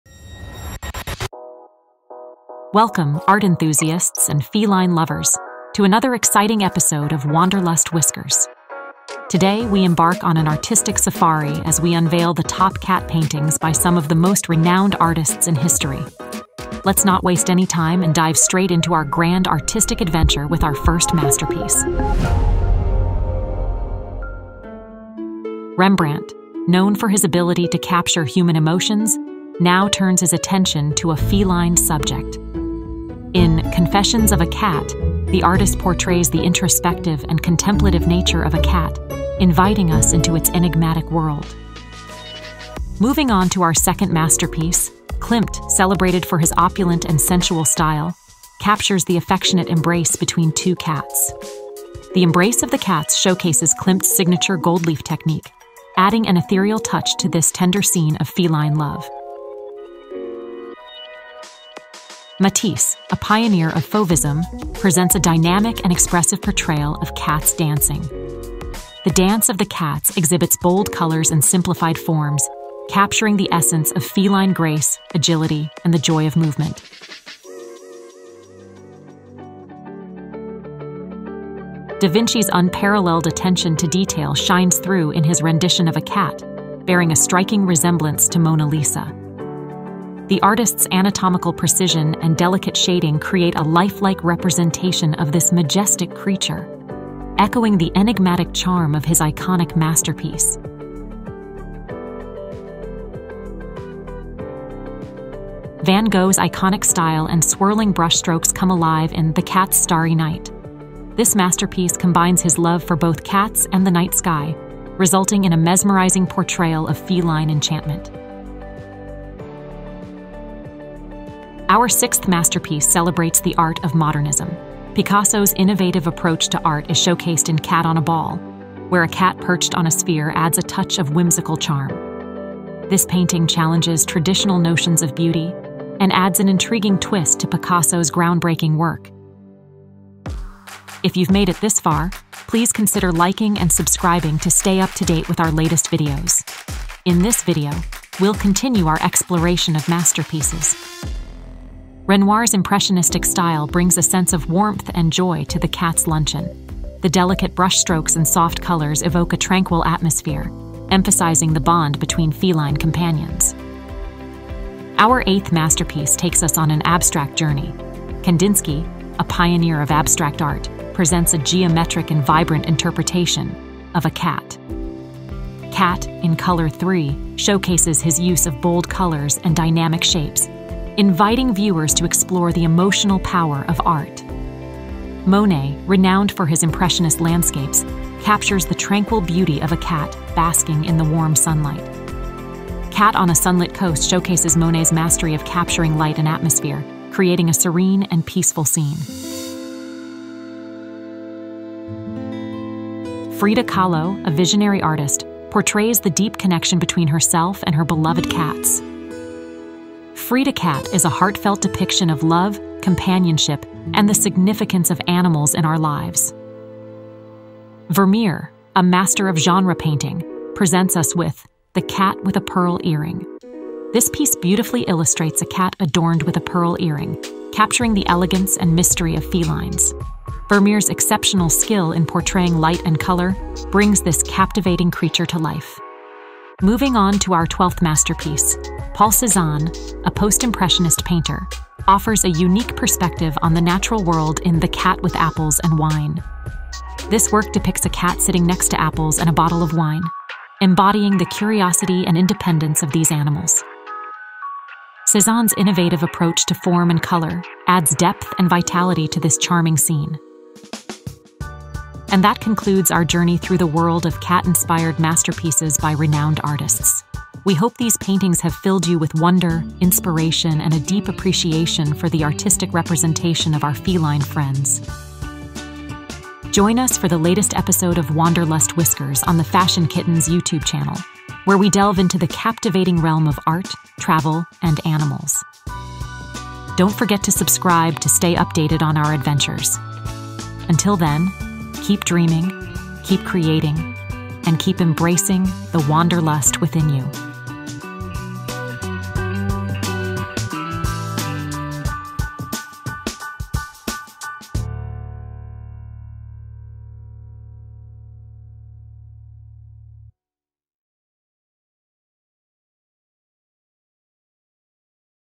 Pawtraits of Greatness: Cats Inspire the Masters | Wanderlust Whiskers
Embark on an artistic safari as we unveil the top cat paintings by renowned artists like Rembrandt, Gustav Klimt, Henri Matisse, Leonardo da Vinci, Vincent Van Gogh, Picasso, Renoir, Wassily Kandinsky, Monet, Frida Kahlo, Vermeer, Paul Cézanne.

From realistic portraits to abstract interpretations, these feline-inspired masterpieces will whisker your imagination away! Join us on this captivating journey of art and cats!